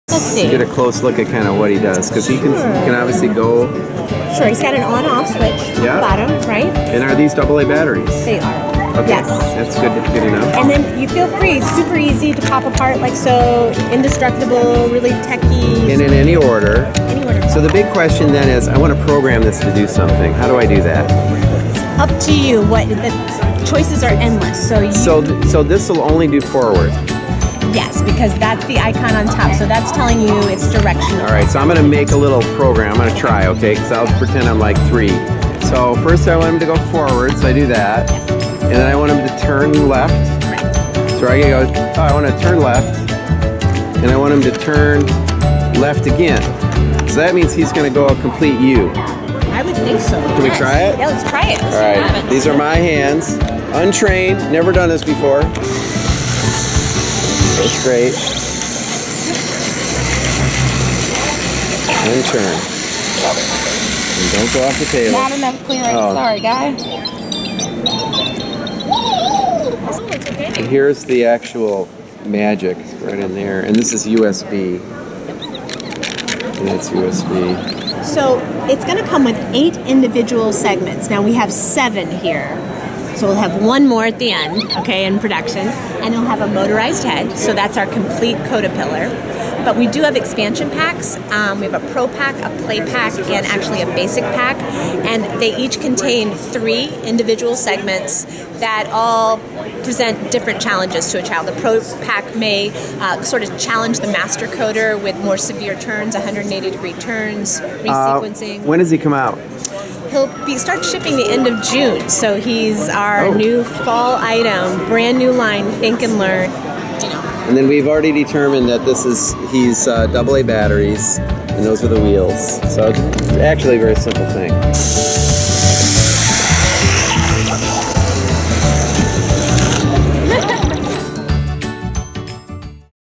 Think & Learn Code-A-Pillar from Fisher-Price
Code-A-Pillar from Fisher-Price is a self propelled, snap-together toy that can be controlled by re-arranging  links. Each link has a USB-like connection. To execute your program, you push a "start" button on the bugs head. Powered by AA batteries, Codepillar will be out in June 2016. Full details at ctrex.us